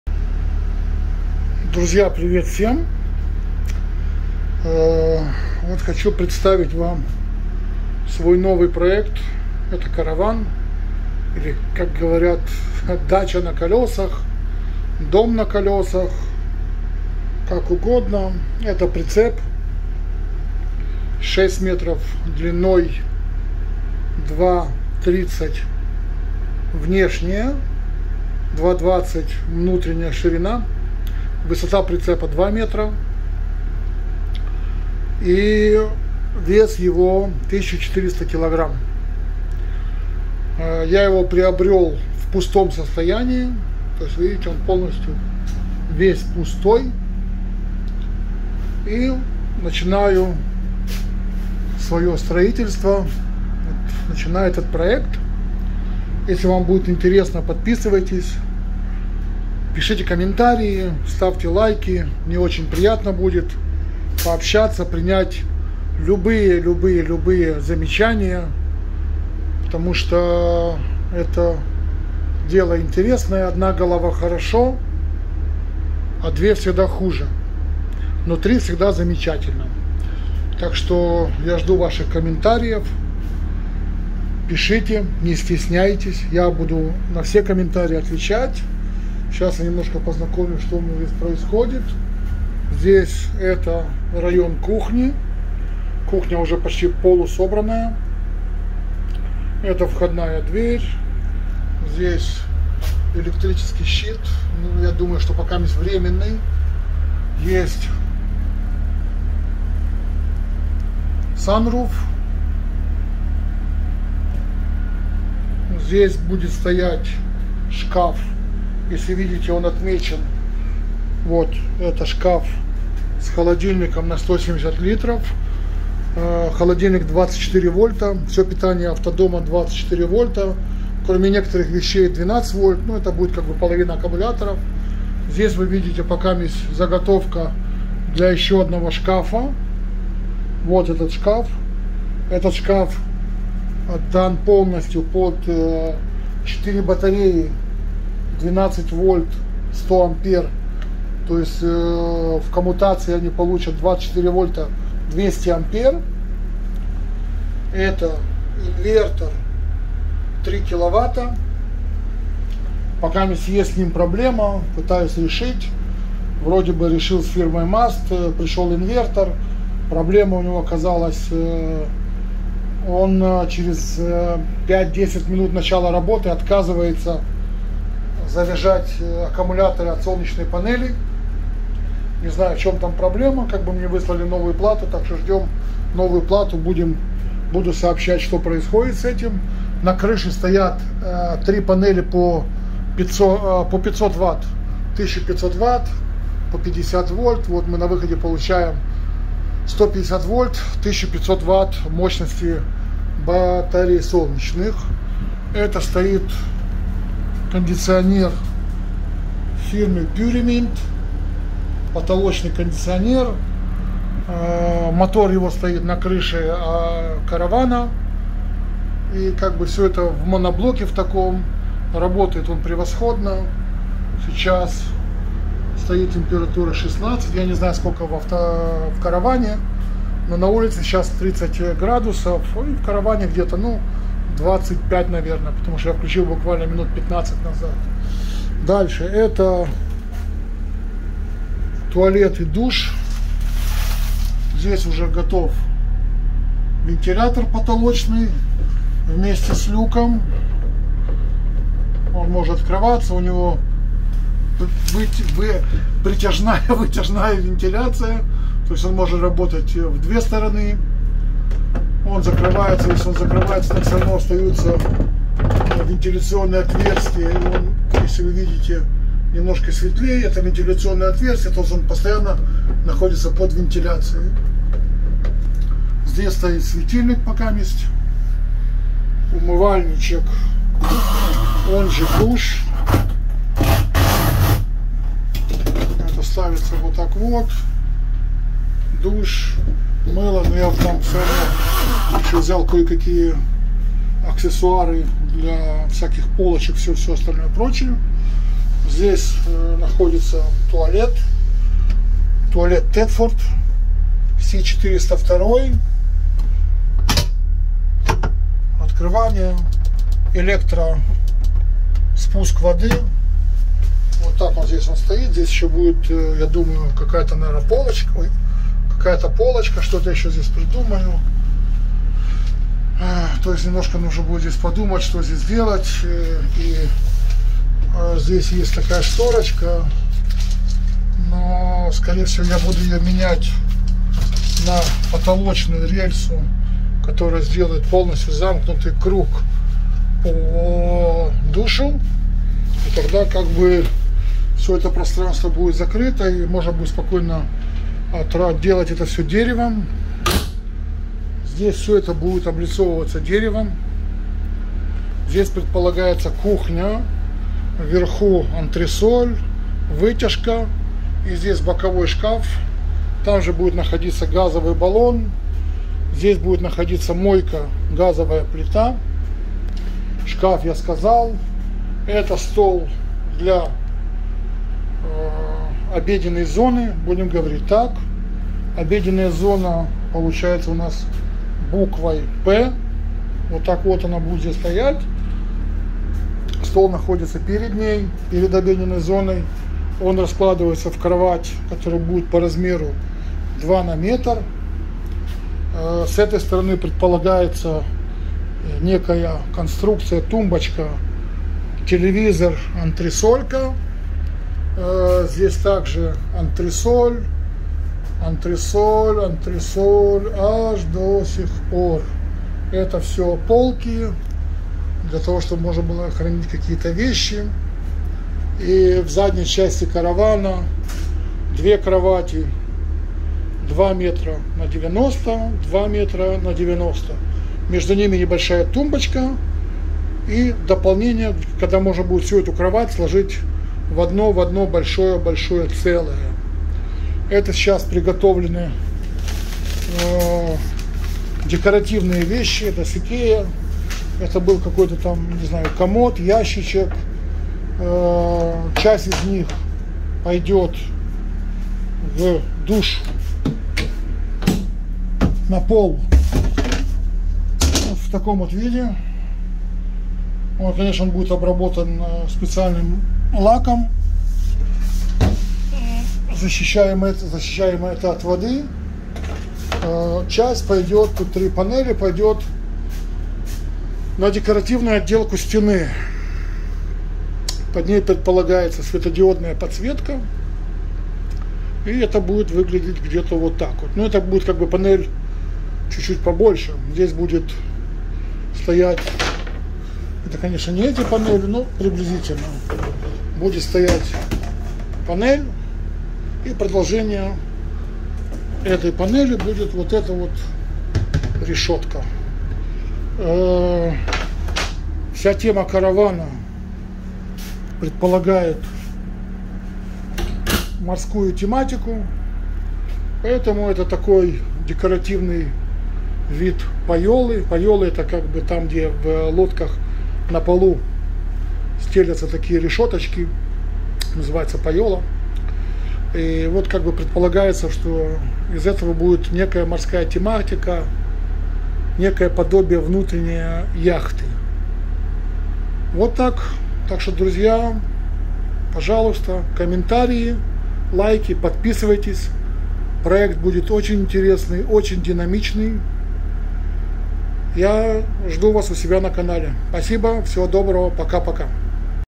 Caravan - Yacht. Караван - яхта.
Проэкт постройки прицепа - дачи с элементами декора яхты.  Длина прицепа 6м, ширина 2.3м, высота внутри 2м.